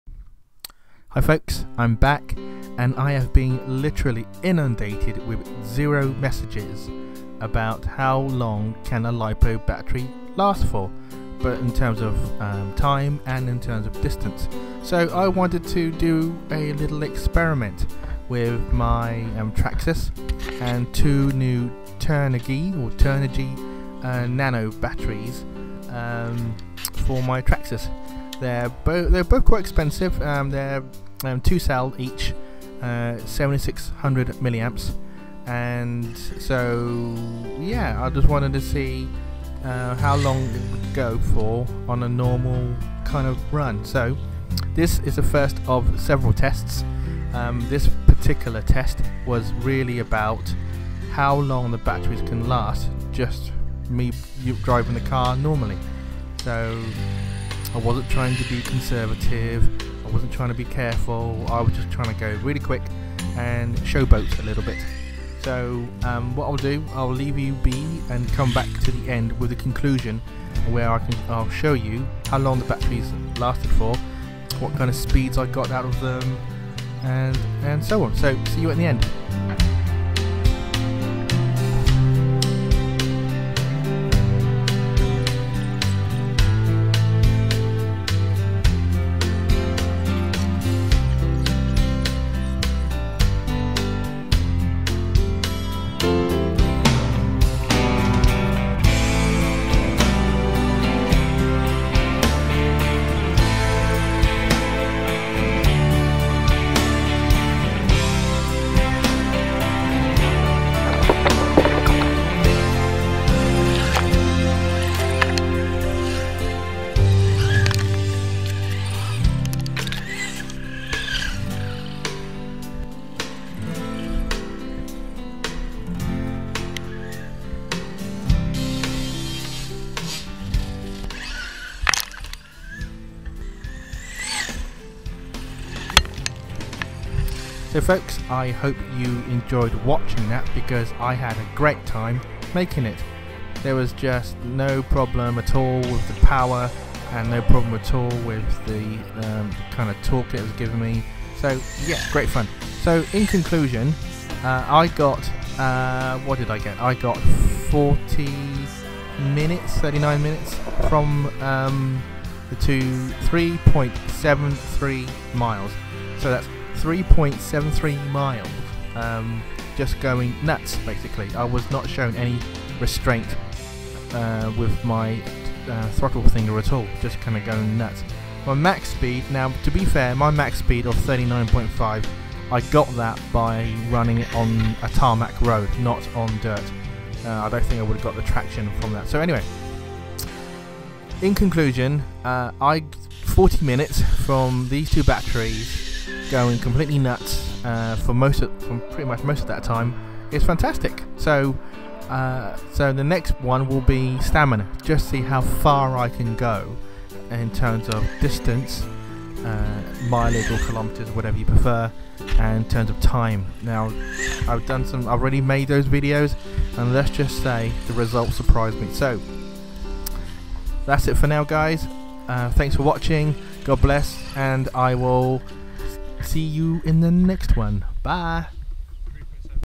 How long do Turnigy Nano-tech batteries last for?
I have always wondered just how good are these expensive parts of the RC Hobby that not many people discuss in terms of stamina? So with my trusty Traxxas Summit I hook up a GPS unit to see how fast, how far and how long the Turnigy LIPO batteries will go.